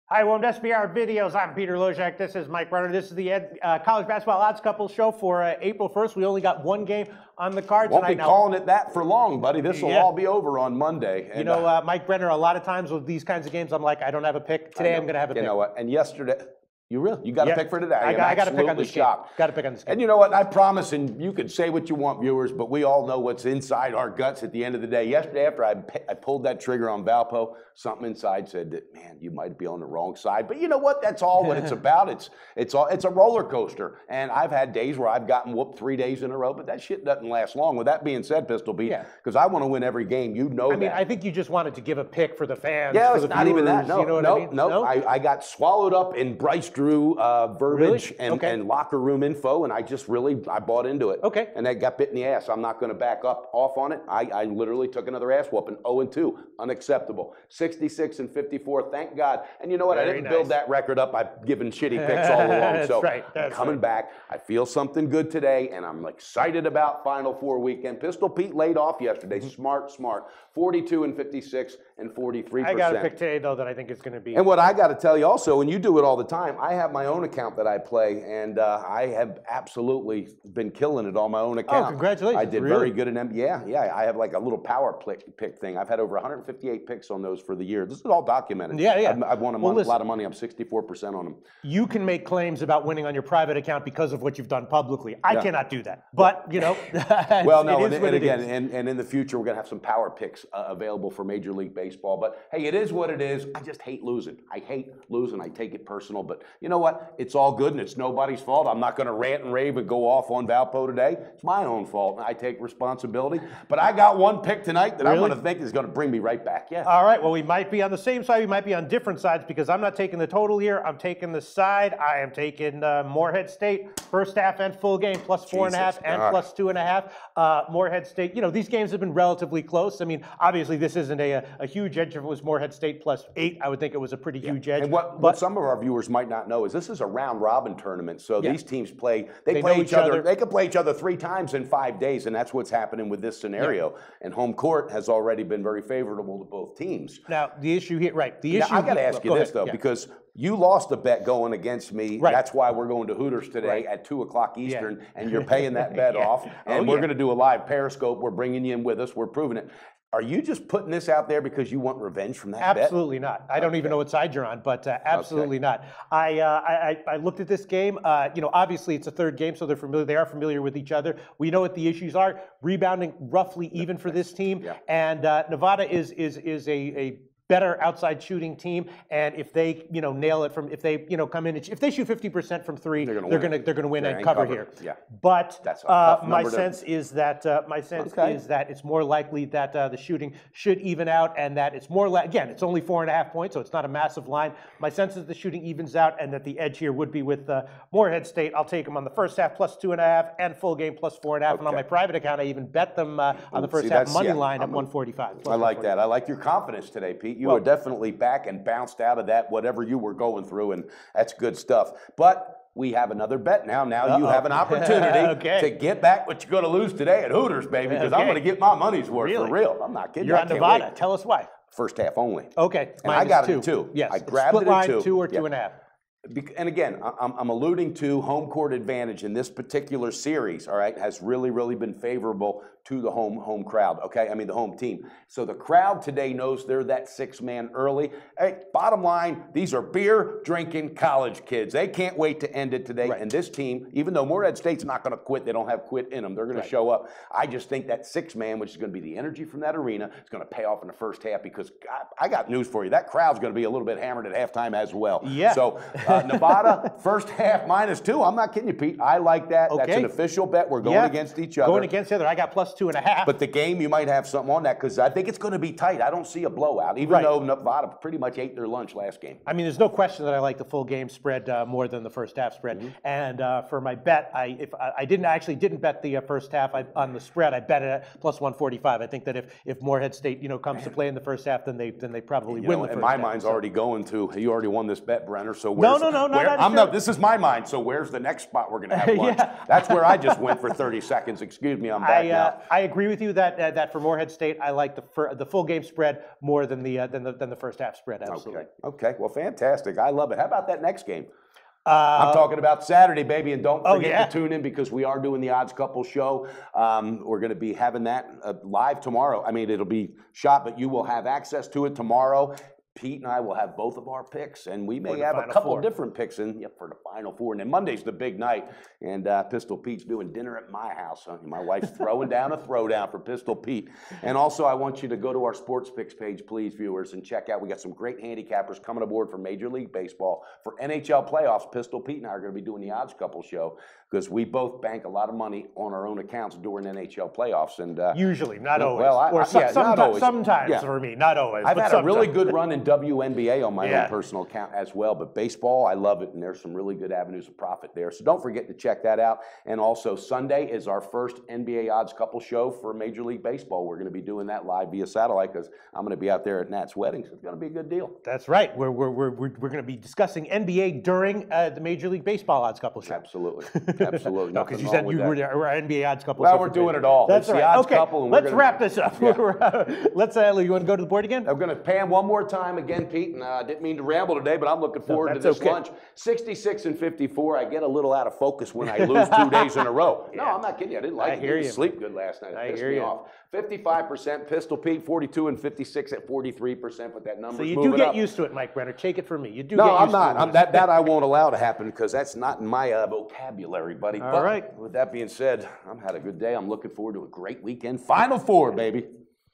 NCAA Basketball Betting: April Fools? Must Be Why We're Getting Such Great Value Picks!
After splitting their first 2 games in the CBI finals, Morehead St and Nevada face off in the rubber match tonight. SBR Videos hosts Mike Brenner and Peter Loshak present their college basketball Odds Couple show for Friday April 1.